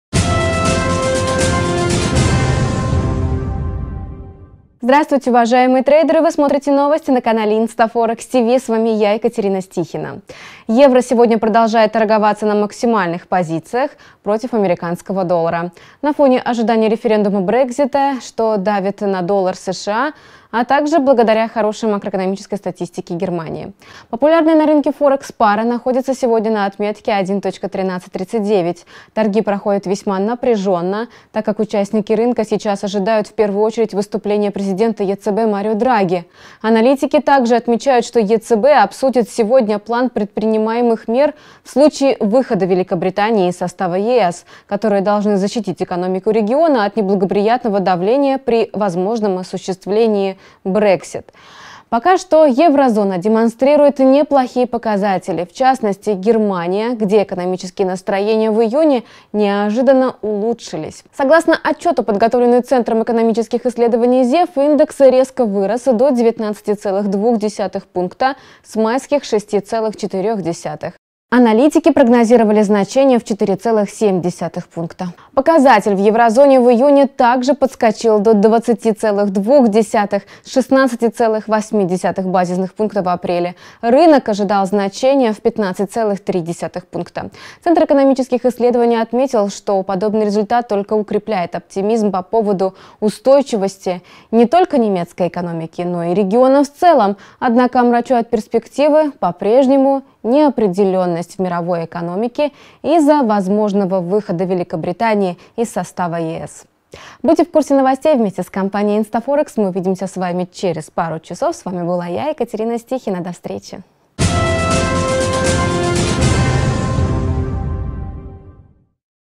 Пара EUR/USD торгуется на максимальных позициях
Евро сегодня продолжает торговаться на максимальных позициях против американского доллара на фоне ожиданий референдума Brexit, что давит на доллар США, а также благодаря хорошей макроэкономической статистике Германии.
Популярная на рынке Форекс пара находится сегодня на отметке 1.1339. Торги проходят весьма напряженно, так как участники рынка сейчас ожидают в первую очередь выступления президента ЕЦБ Марио Драги.
Аналитики также отмечают, что ЕЦБ обсудит сегодня необходимые меры в случае выхода Великобритании из состава ЕС, которые должны защитить экономику региона от неблагоприятного давления. Пока что еврозона демонстрирует неплохие показатели. В частности Германия, где экономические настроения в июне неожиданно улучшились.
Согласно отчету, подготовленному центром экономических исследований ZEW, индекс экономических исследований резко вырос до 19,2 пункта с майских 6,4.  Аналитики прогнозировали меньший рост — до 4,7 пункта.
Показатель в еврозоне в июне также подскочил до 20,2 с 16,8 базисных пункта в апреле. Рынок ожидал роста до 15,3 пункта.
Центр экономических исследований отметил, что подобный результат только укрепляет оптимизм по поводу устойчивости не только немецкой экономики, но и региона в целом. Однако по-прежнему омрачает перспективы неопределенность в мировой экономике и возможные итоги референдума Brexit.